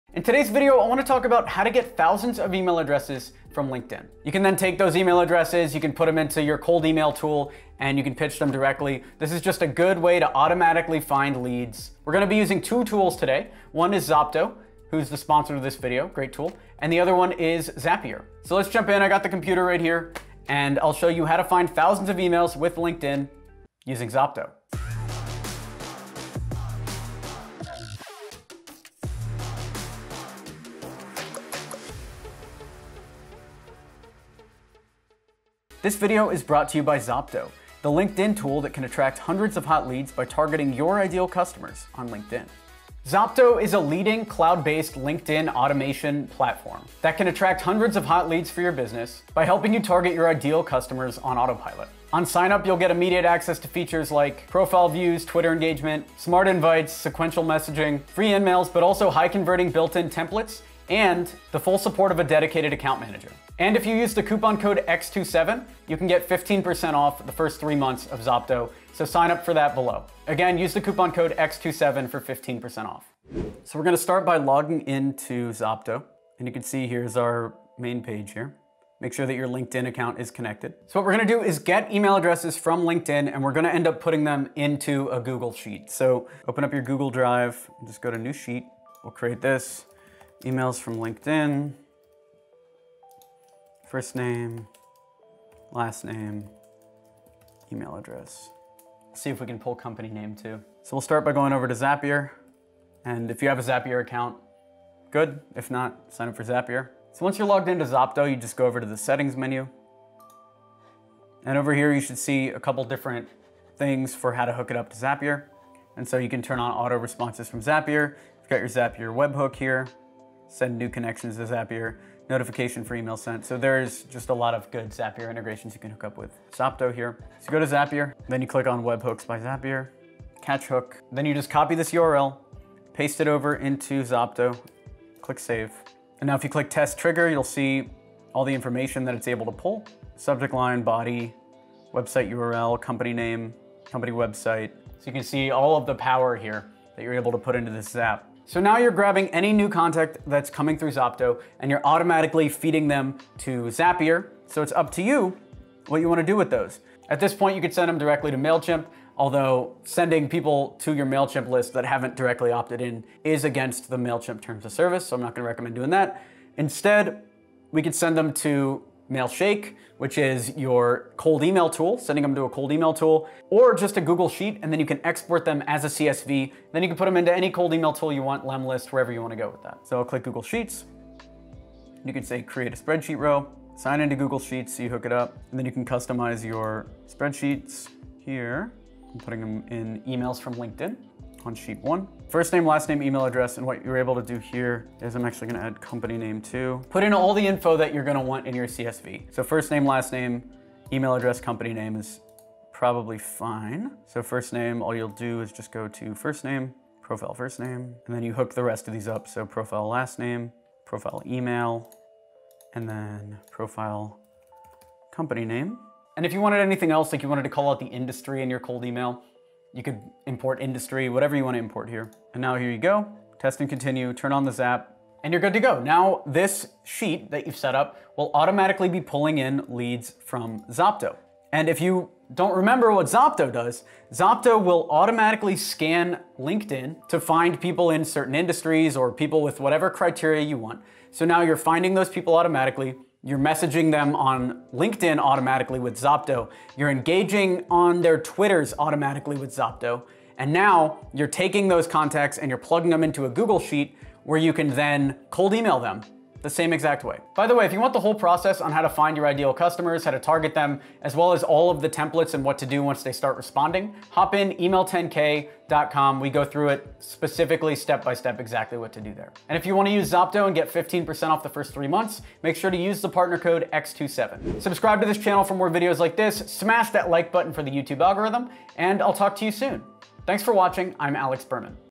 How to Get Thousands of Email Addresses from LinkedIn with Zopto
In today's video, I want to talk about how to get thousands of email addresses from LinkedIn.

Want to learn how to get clients that pay 10X more? Hop on in at ➡️

You can then take those email addresses, you can put them into your cold email tool, and you can pitch them directly.

This is just a good way to automatically find leads.

We're going to be using two tools today - one is Zopto, who's the sponsor of this video - great tool, and the other one is Zapier.

So let's jump in, I got the computer right here, and I'll show you how to find thousands of emails with LinkedIn using Zopto.

Zopto's automation tools are mainly used by sales teams to automate their LinkedIn lead generation efforts with features such as Profile Views, Twitter Engagements, Smart Invites, Free InMails, and Sequential Messaging.